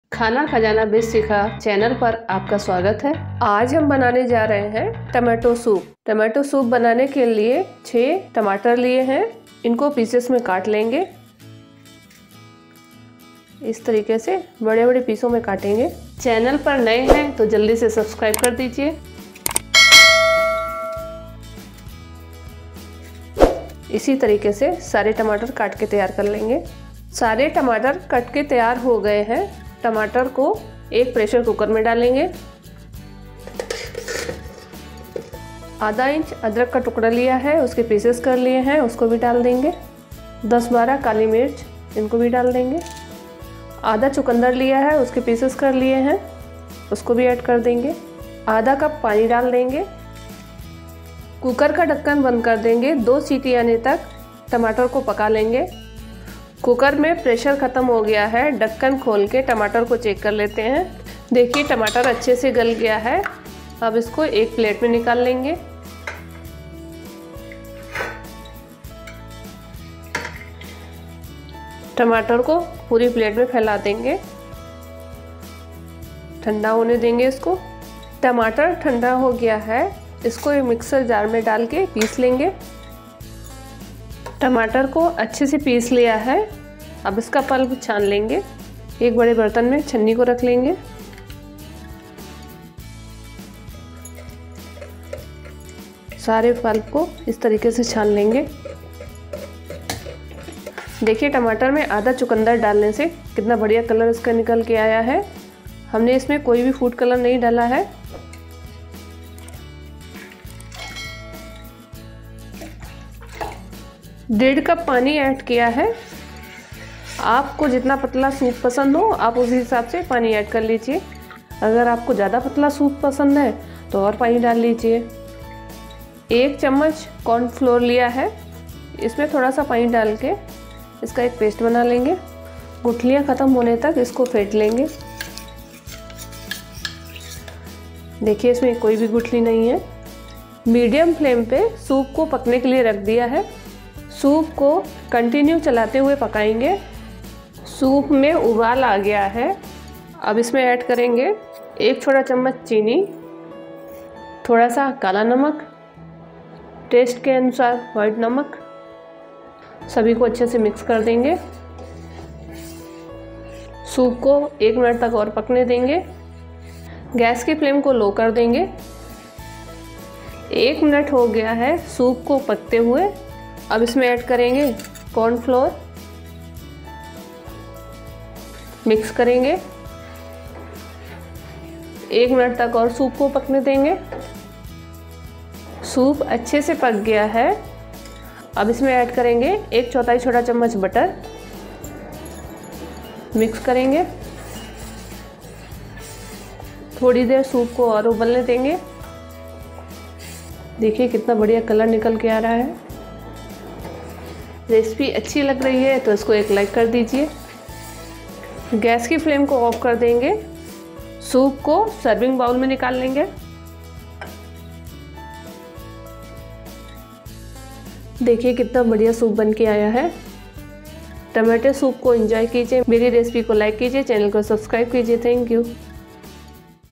टमाटर का सूप कैसे बनता है | Tamatar ka Soup without added colour | Tomato Soup | Best Soup Recipes
टमाटर का सूप कैसे बनाएं (Tomato Soup / Soup Recipes): घर पर बने टोमेटो सूप की बात ही अलग है और इसे बनाने में भी कोई झंझट नहीं है| इस सूप को आप सर्दी और गर्मी दोनों ही मौसम में पी सकते हैं।
आइए जानें टमाटर का सूप कैसे बनता है (टमाटर का सूप बनाने की विधि) |

Please watch the whole video Tomato Soup Recipe in Hindi and leave a feedback in Comments.